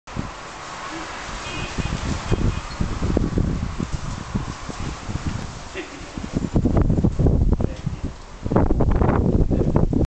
赤い砂嵐＠アフリカ Red sand storm@Africa
豪州で、赤い砂嵐が起こった報道がありましたが、
私も08年末アフリカで体験しました。

I saw this exciting sand storm in Dec 08 in Africa.
After that I heard this happened in Australia also.